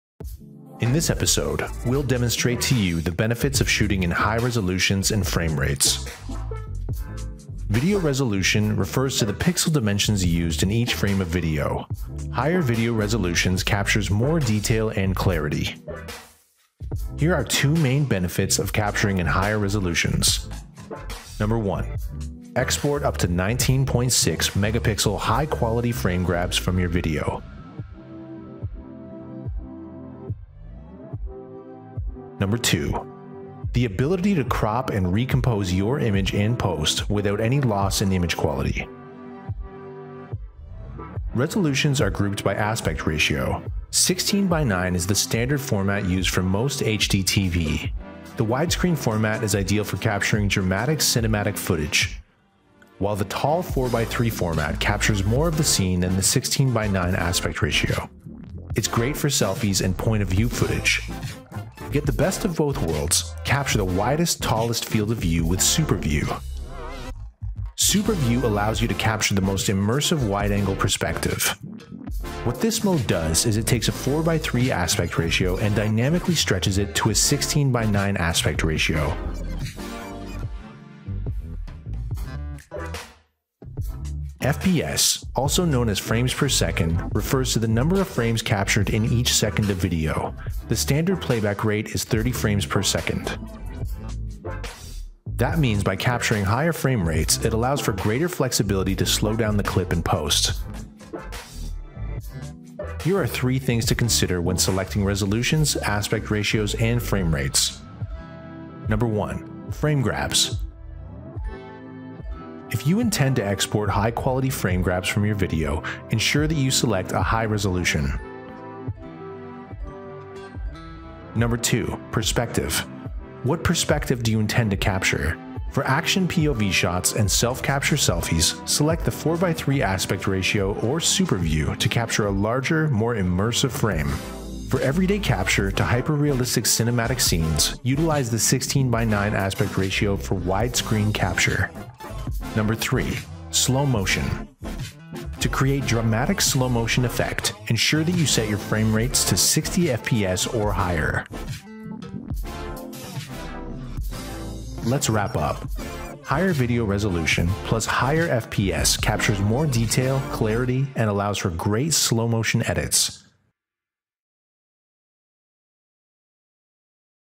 GoPro HERO10: How to Choose the Best Resolution + Frame Rate
See how the right resolution, frame rate and aspect ratio can help you do more with your GoPro video. Grab an ultra high-quality 19.6MP photo from your video, crop your videos without losing any image quality, slow down the action and more.​